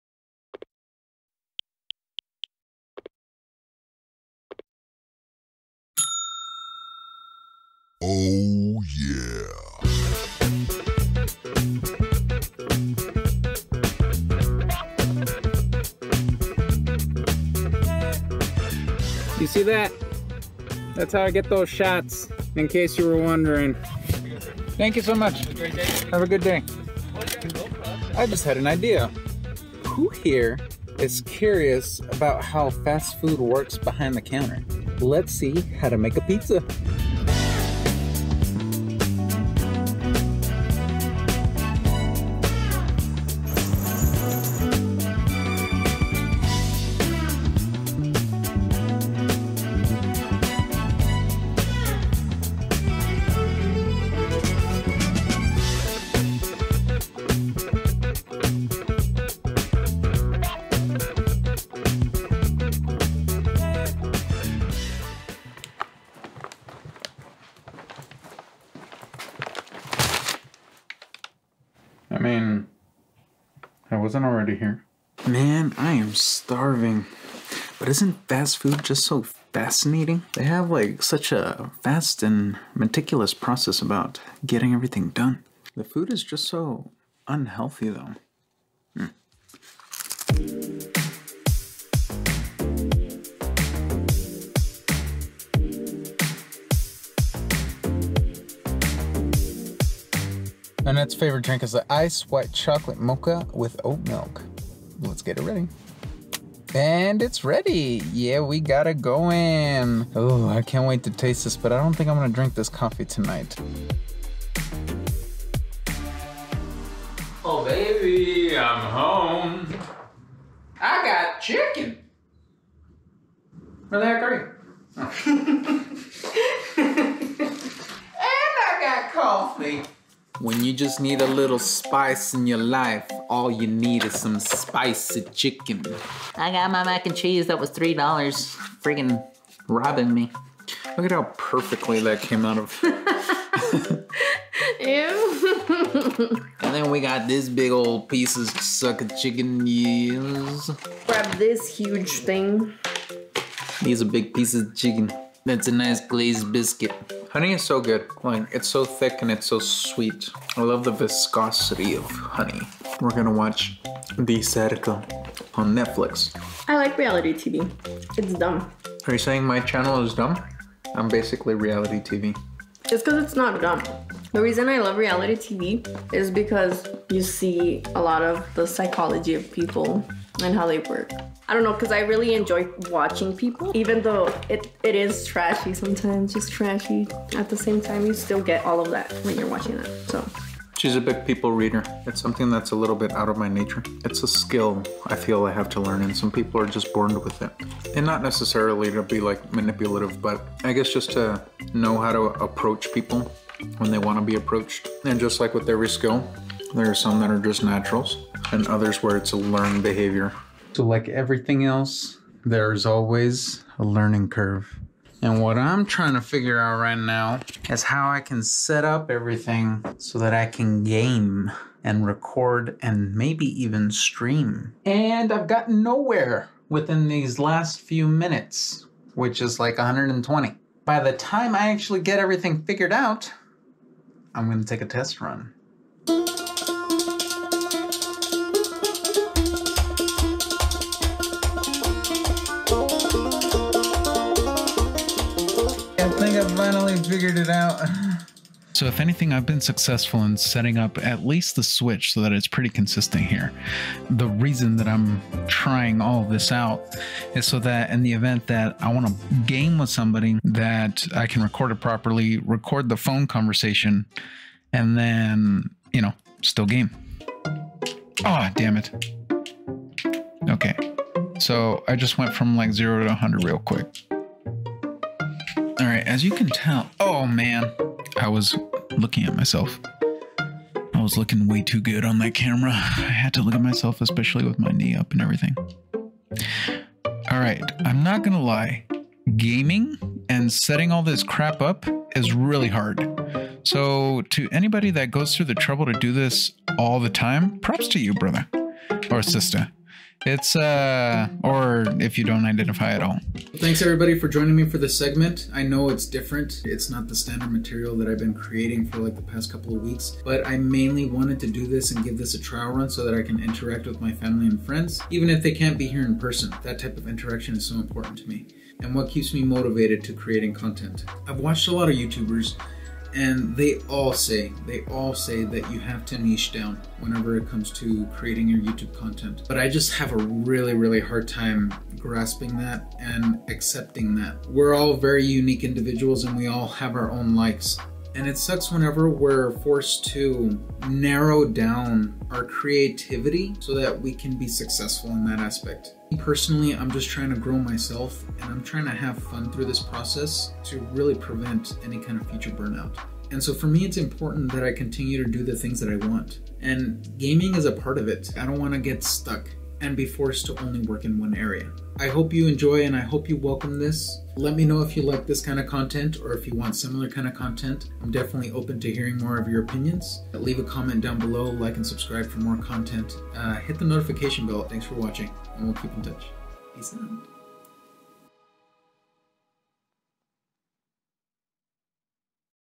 Making a Fast Food Pizza🍕 | Daily Vlog
In this episode we go behind the scenes learning how to make a fast food pizza. We explore different creative areas in my daily vlog world, enjoy a meal with my wife and I, and hang out!